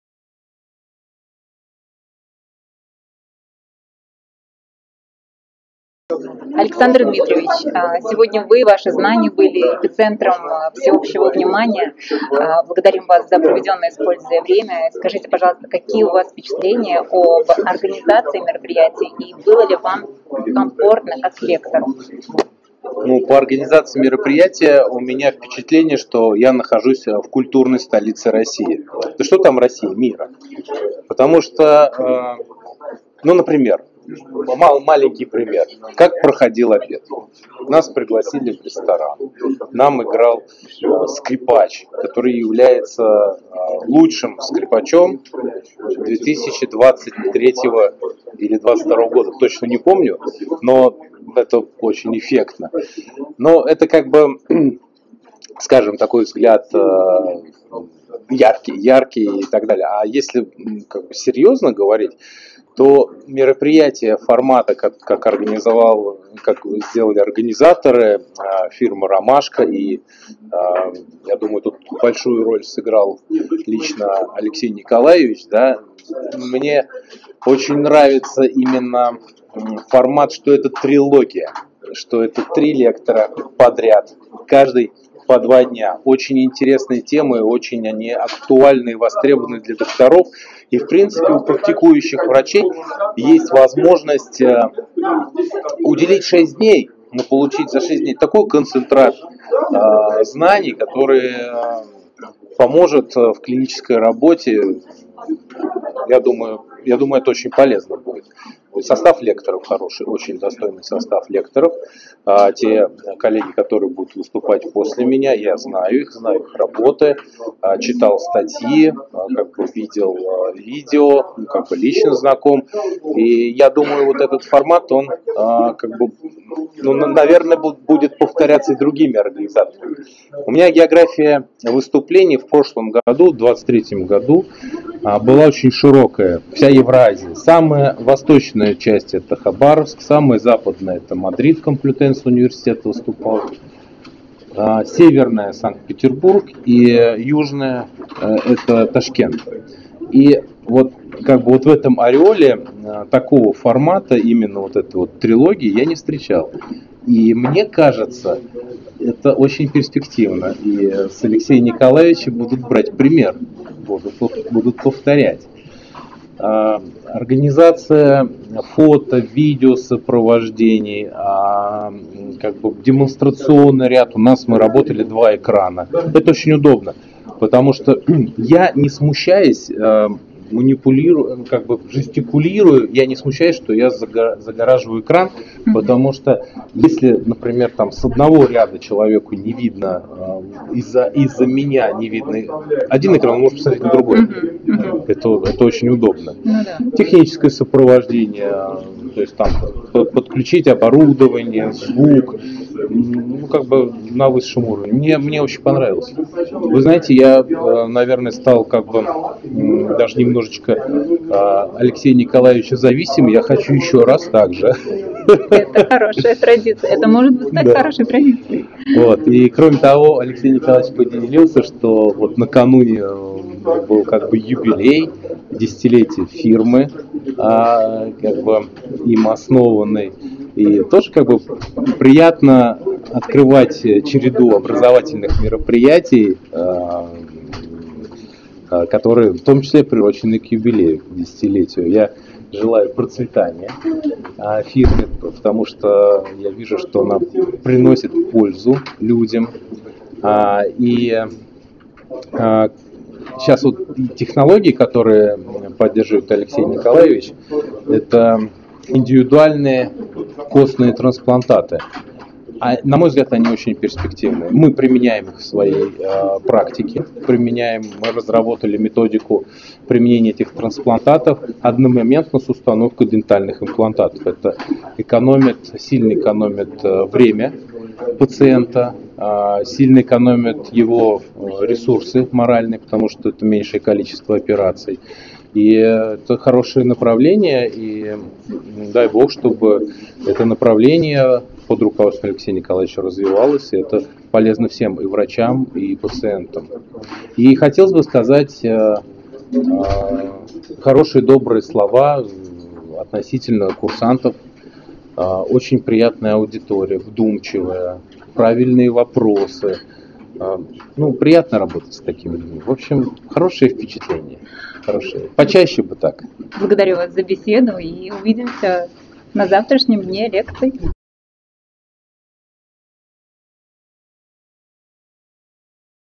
Александр Дмитриевич Лысов. Хирургическая трилогия 2024. Отзыв лектора №18
Практический семинар Александр Дмитриевич Лысов
Темы:" Пластика мягких тканей в периимплантной зоне", " Одномоментная имплантация с пластикой мягких тканей", " Индивидуальные заживляющие абатменты (ИЗА)"
30 марта - 4 апреля 2024
«Хирургическая трилогия» в Санкт-Петербурге!
3 звездных лектора: Виталий Панцулая, Александр Лысов, Мария Носова.
6 дней непрерывного погружения в продвинутую пластическую хирургию мягких тканей и костную пластику. Демонстрации, мастер-классы, практические занятия.